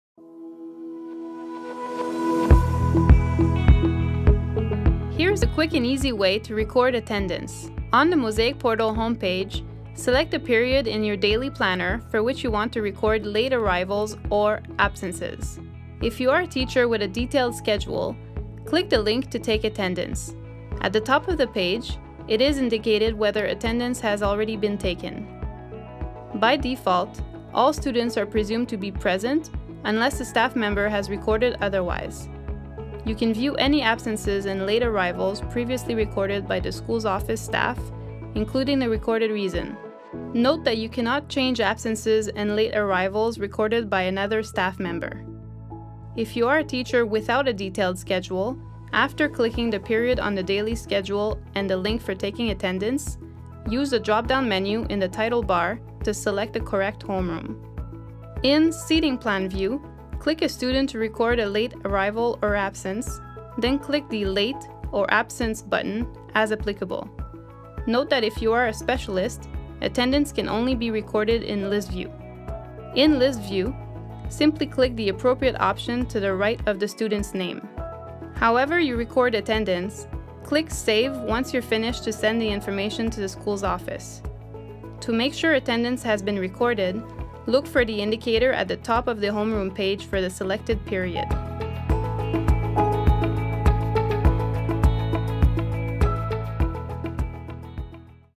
Record Attendance - Mozaïk-Portal For Teachers (Elementary school)
**Warning** : The functionalities presented in this capsule may differ from those of your school due to software updates or the lack of activation of certain functionalities by your school organization.

Mozaïk-Portal provides a quick and easy way to record attendance.